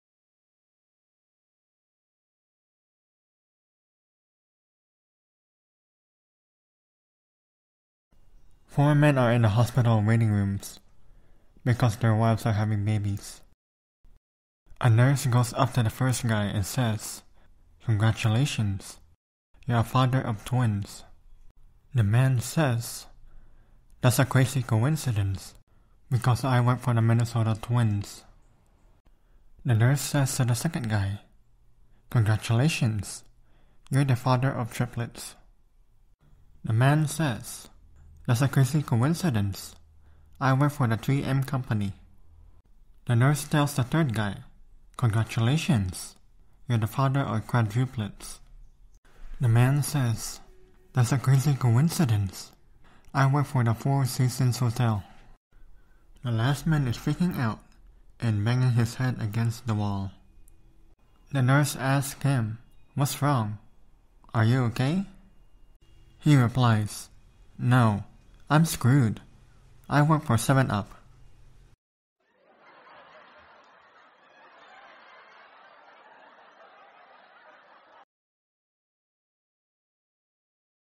Super Funny (Father) Joke: 4 men are in the hospital waiting rooms because their
😂 Daily Super Funny Jokes 😂

4 men are in the hospital waiting rooms because their wives are having babies.

A nurse goes up to the first guy and says, “Congratulations, you’re a father of twins.”

The man says, “That’s a crazy coincidence, because I work for the Minnesota Twins.”

The nurse says to the second guy, “Congratulations, you’re the father of triplets.”

The man says, “That’s a crazy coincidence, I work for the 3M Company.”

The nurse tells the third guy, “Congratulations, you’re the father of quadruplets.”

The man says, “That’s a crazy coincidence, I work for the four seasons hotel.”

The last man is freaking out and banging his head against the wall.

The nurse asks him, “What’s wrong?

He replies, “No, I’m screwed! I work for 7UP.”